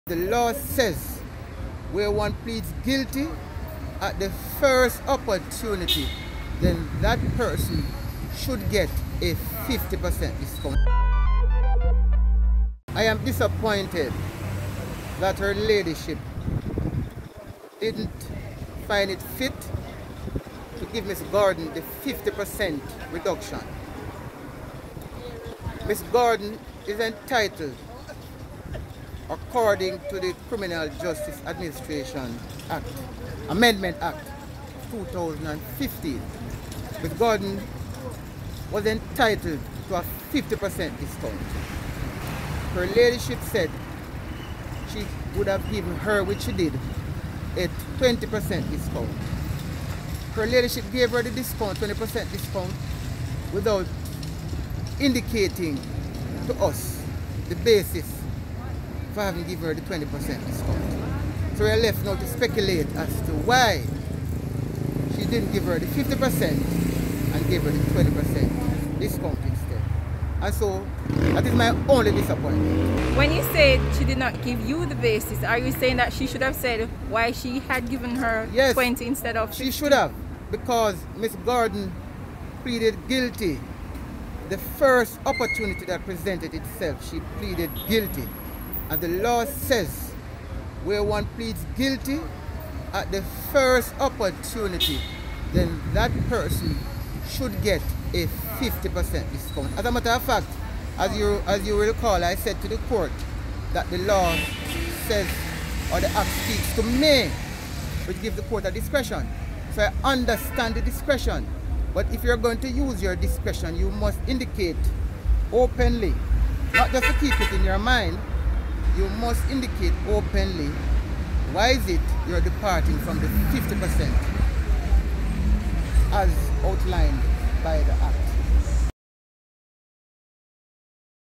Former NCB manager imprisoned for seven and half years
The former senior manager at the National Commercial Bank (NCB) who admitted to swindling $34 million from the financial institution was this morning sentenced to a total of seven years and six months in prison.